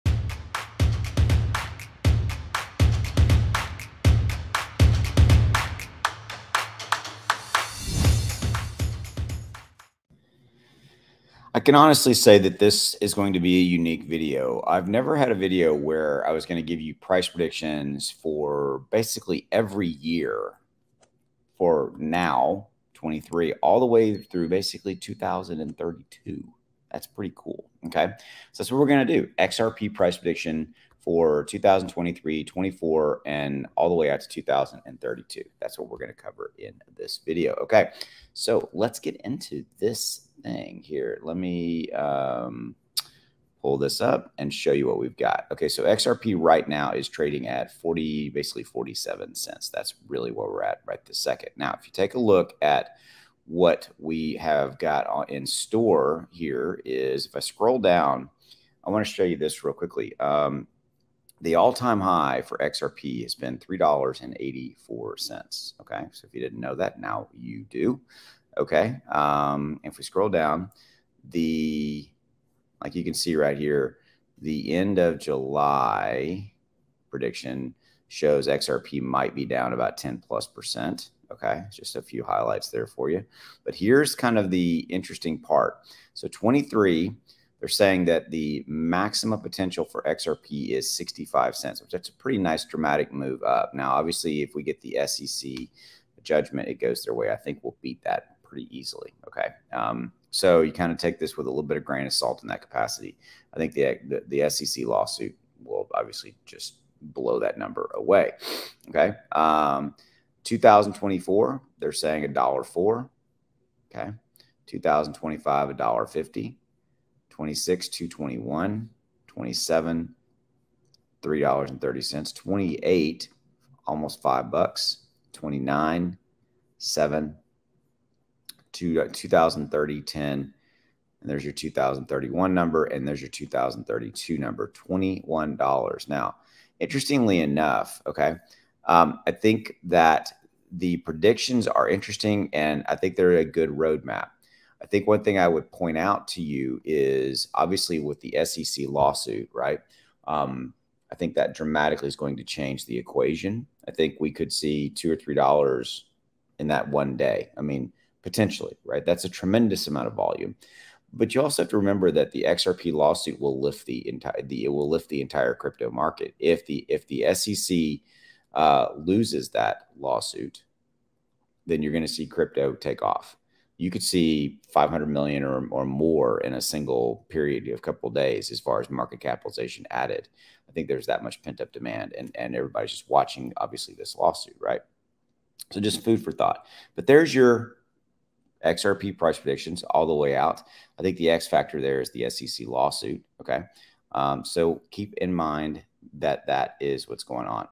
Revealing This MASSIVE Ripple XRP Price Prediction up to 2030!!!
This is a massive Ripple XRP price prediction through until 2030. Let me know if you agree with this prediction.